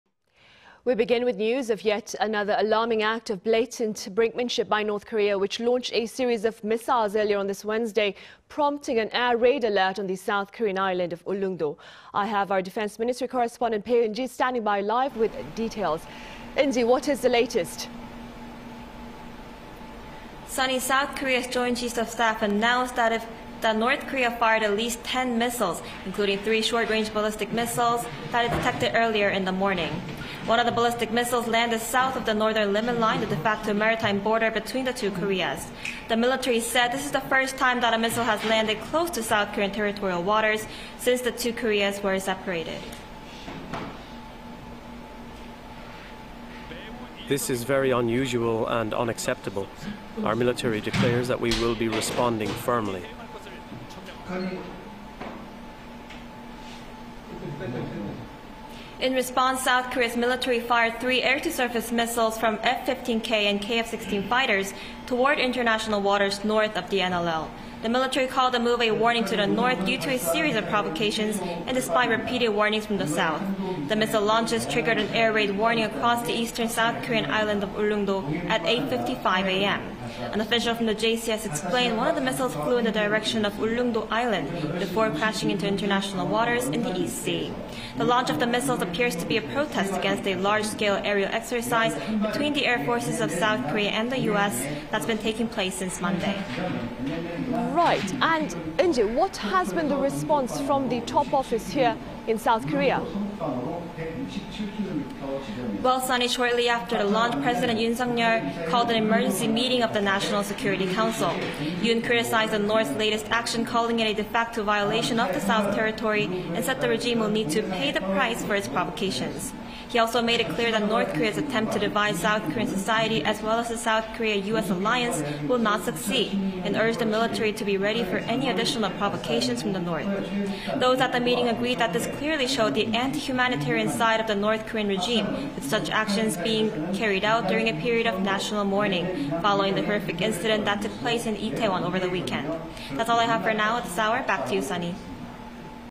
N. Korean ballistic missile lands near S. Korea's territorial waters for the first time
北, 사상첫 NLL이남 탄도미사일 도발·다른 방향 10여발 발사

We begin... with news... of yet another alarming act... of blatant brinkmanship... by North Korea... which launched... a series of missiles... earlier... on this Wednesday... prompting an air raid alert... on the South Korean island of Ulleungdo .
I have... our Defense Ministry Correspondent... Bae Eun-ji  standing by... live... with details.
Eun-ji ... what is the latest?

Sunny, South Korea's Joint Chiefs of Staff said North Korea fired at least ten missiles, including three short-range ballistic missiles that it detected earlier in the morning.
One of the ballistic missiles landed south of the Northern Limit Line, the de facto maritime border between the two Koreas.
The military said this is the first time that a missile has landed close to South Korean territorial waters -- since the two Koreas were separated.

"This is very unusual and unacceptable. Our military declares that we will be responding firmly."

In response, South Korea's military fired three air-to-surface missiles from F-15K and KF-16 fighters, toward international waters north of the NLL.
The military called the move a warning to the North,... due to a series of provocations and despite repeated warnings from the South.
The missile launches triggered an air raid warning across the eastern South Korean island of Ulleung-do at 8:55 AM.
An official from the JCS explained one of the missiles flew in the direction of Ulleung-do Island,... before crashing into international waters in the East Sea.
The launch of the missiles appears to be a protest against a large-scale aerial exercise between the air forces of South Korea and the U.S. -- that's been taking place since Monday.

Right... and... Eun-ji ... what has been the response... from the top office... here?

Well Sunny... shortly after the launch, President Yoon Suk-yeol called an emergency meeting of the national security council.
Yoon criticized the North's latest action, calling it a de facto violation of the South's territory,... and said the regime will need to "pay the price" for its provocations.
He also made it clear that North Korea's attempt to divide South Korean society as well as the South Korea-U.S. alliance... will not succeed... and urged the military to be ready for any additional provocations from the North.
Those at the meeting agreed that this clearly showed the anti-humanitarian side of the North Korean regime,... with such actions being carried out during a period of national mourning -- following the horrific incident that took place in Itaewon over the weekend.
That's all I have for now at this hour.
Back to you, Sunny.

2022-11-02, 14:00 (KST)